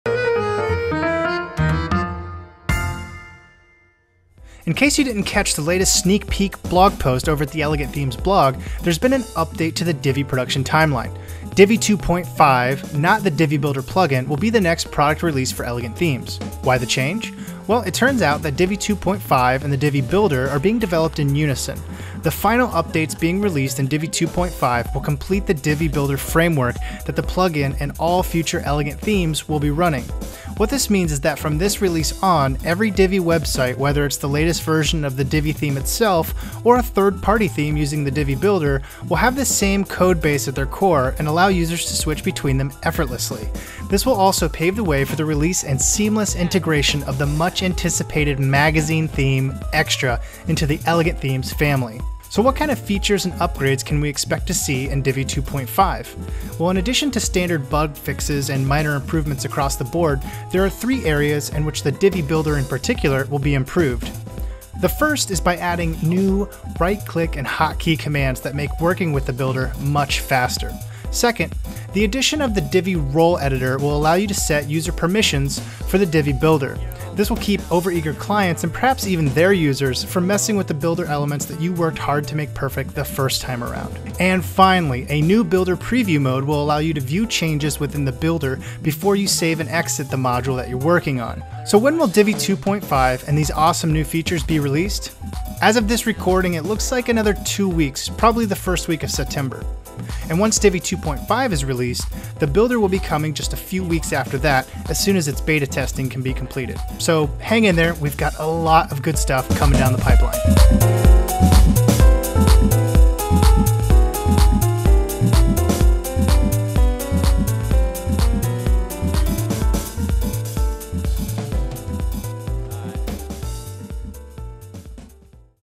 Elegant Themes Product Update: New Features Coming in Divi 2.5
In this Elegant Themes Product Update we talk about the updated production timeline of the Divi Builder Plugin and Divi 2.5. We also go over the three key new features they'll have in common.